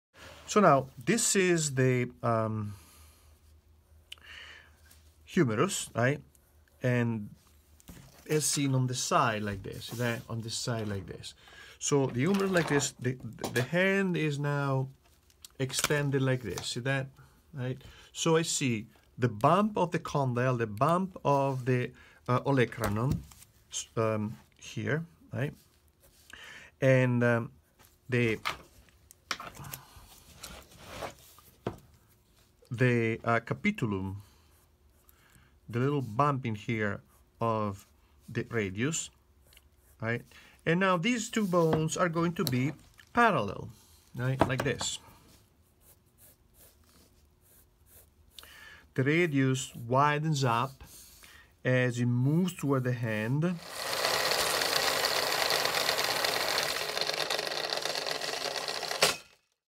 Elbow Joint And Bones: Understanding Figure Anatomy
In this short we are describing the anatomy of the elbow joint and the bones involved in that area. The condyle and ulna bumps, along with the capitulum and radius, are key features of the elbow structure. The parallel alignment of the radius and ulna, with the radius widening towards the hand, highlights the intricate design of our skeletal system. Understanding these details can be helpful for medical professionals, students studying anatomy, or anyone interested in the fascinating complexity of the human body.